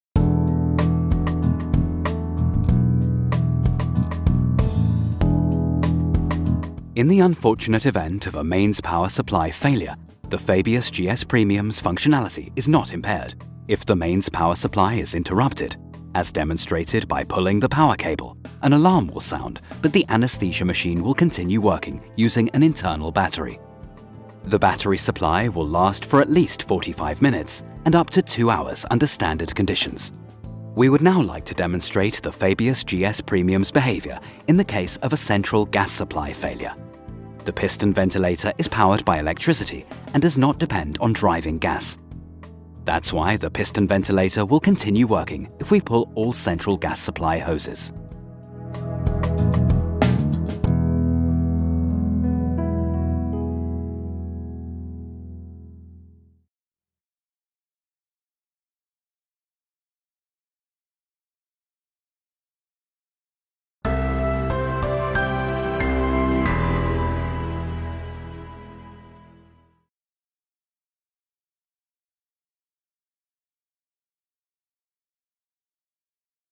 Good to know about Fabius GS premium - Safety Features
Watch why you can rely on your Dräger anaesthesia device during power failure or in case of central gas supply failure.